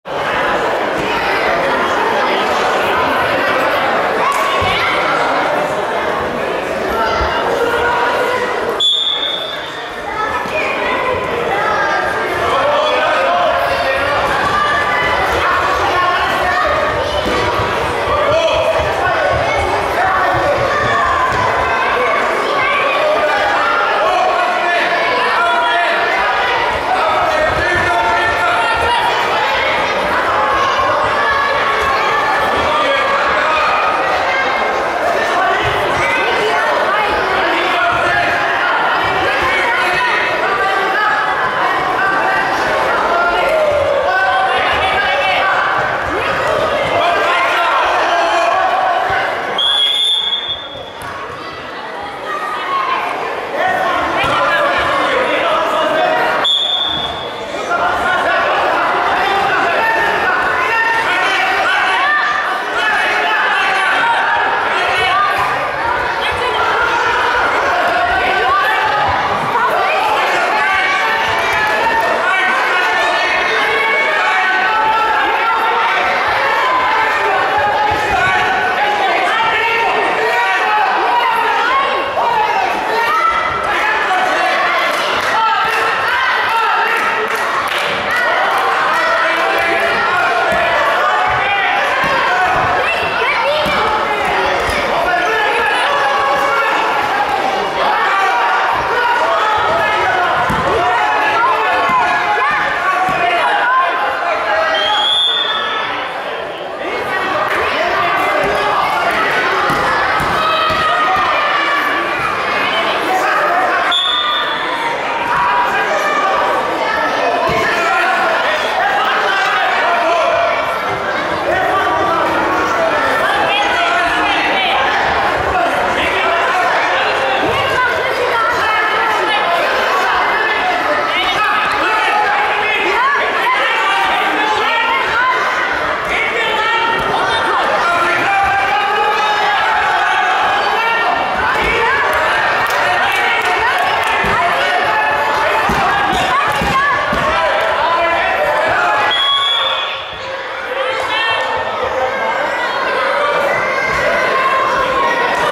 1. Runde Nick Brederlow (SV Halle) - Seyedalireza Khalifeh (RV Hannover) D-Jugend 29kg 63 lbs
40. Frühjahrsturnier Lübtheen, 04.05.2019